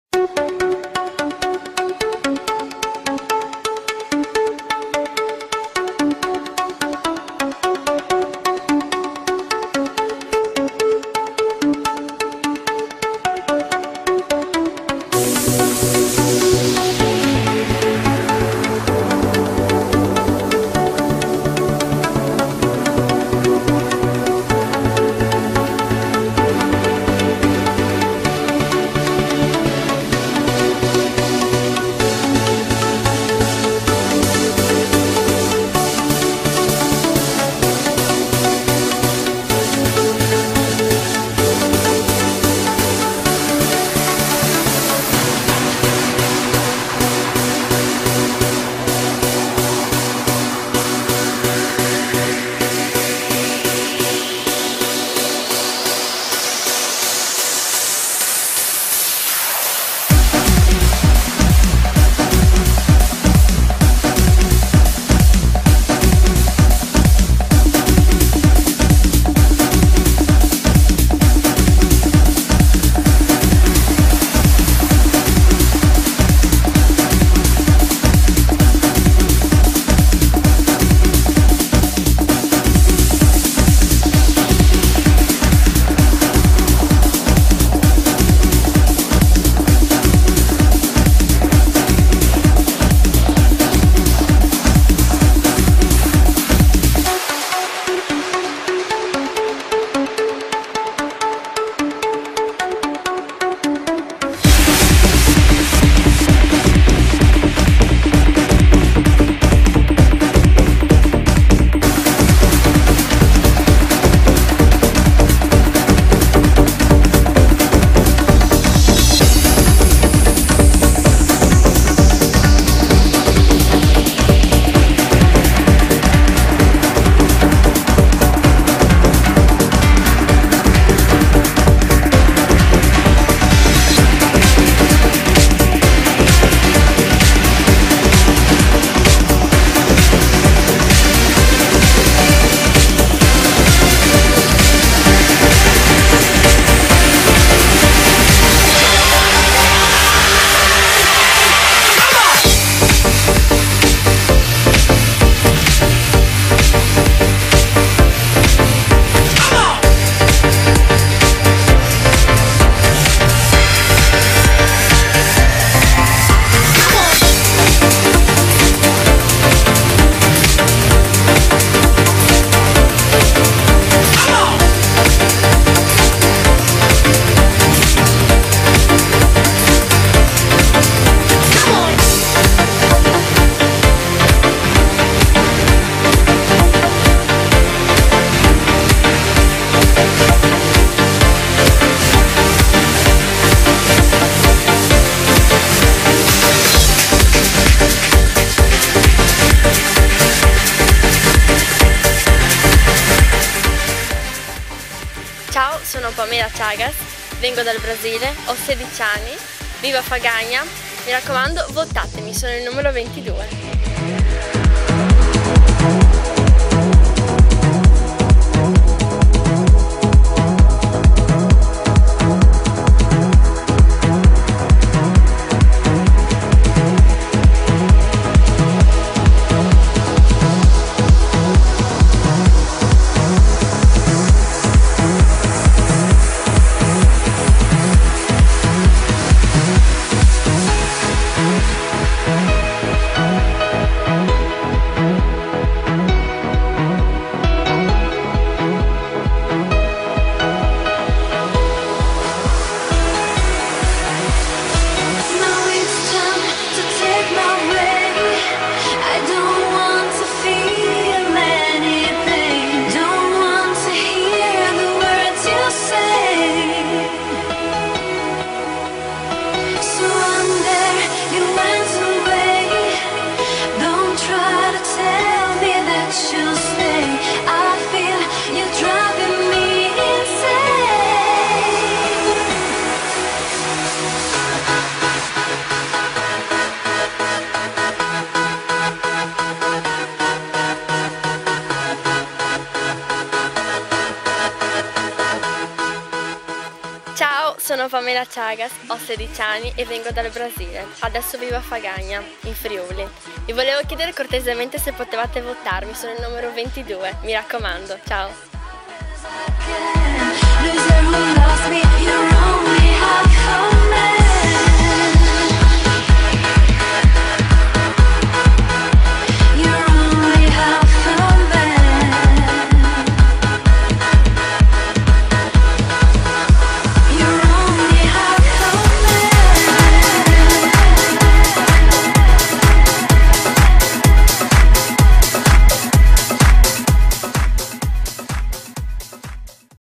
JRS III casting Miss summer Pamela Ciagas 2012 for web
JRS casting  Miss summer 2012  presentazione live della concorrente in riva al mare.
Il web concorso di bellezza creato da Joe Rivetti, casting svolto al Lazzaretto di Muggia TS, III casting, III gruppo, venerdi 17 agosto 2012.
Prodotto da Arthur Spielberg Service, video produzioni digitali in HD.